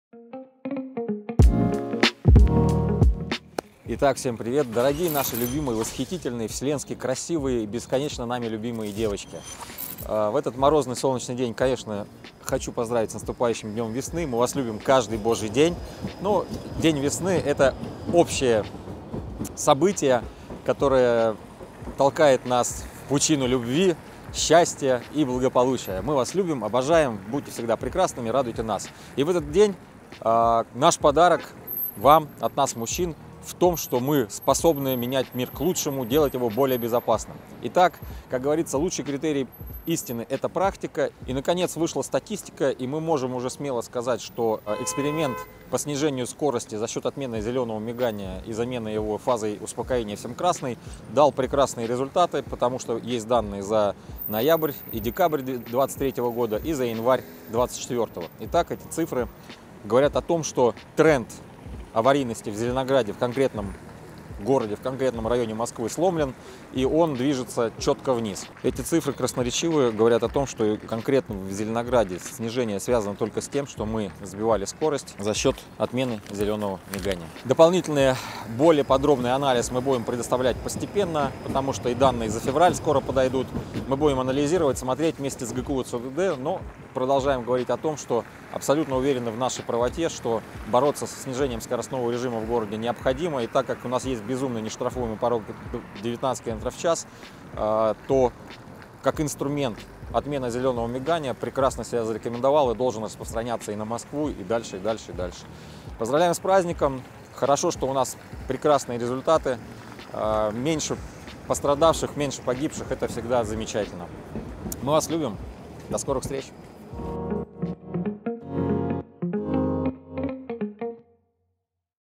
Замена зелёного мигания, новые результаты эксперимента!
Результаты снижения скорости за счёт замены зелёного мигания на фазу успокоения  - лучший подарок нашим восхитительным девочкам на 8 марта!😘😘😘

Лучший критерий истины - это практика!
Концепция нулевой терпимости к смертности в ДТП - это не фантазии.

Анализ статистики аварийности от ГИБДД за 3 месяца эксперимента ГКУ ЦОДД и «СпецДорПроекта» показал, что удалось остановить восходящий тренд травматизма в Зеленограде!

Снижение аварийности (пострадавшие и погибшие) составило 57,78%!!!

Круть!💪💪💪🤟🤟🤟

И как сказал один мой товарищ:
- «Травмы и смерти на дороге - управляемое явление и ГКУ ЦОДД и «СпецДорПроект»  это наглядно ДОКАЗАЛИ.
Против мнения «всезнающей» в дорожном движении толпы».

Будем выдавать анализ статистики дальше, там много интересного для думающих горожан!

Подробности в ролике.

Хорошего дня и Праздника!

К.А.